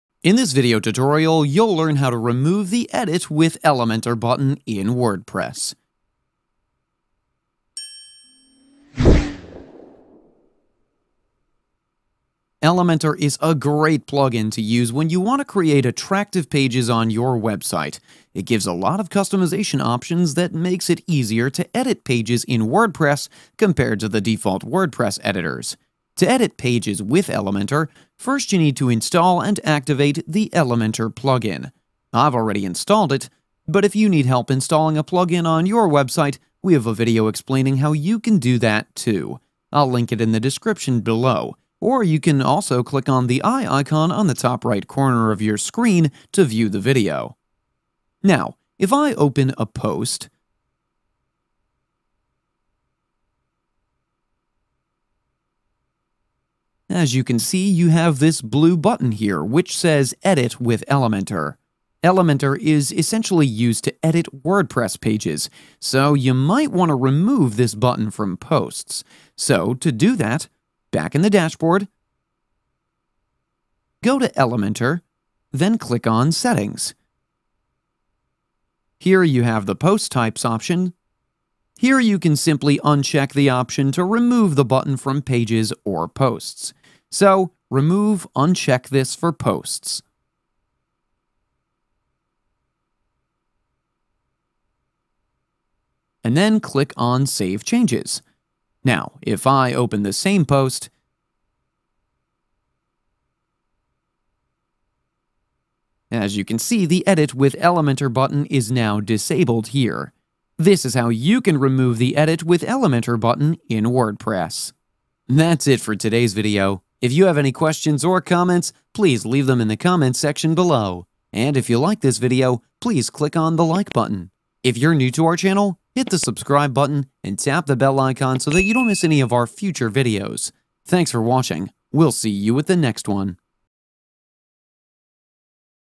How To Remove The Edit With Elementor Button
The easiest way to remove edit With Elementor button. Elementor is a great plugin to use when you want to create attractive pages on your website. It gives a lot of customization options that makes it easier to edit pages in wordpress compared to the default wordpress editors.

0:12 - Introduction.
0:50 - Navigate to To a post
1:10 - Configuring elementor settings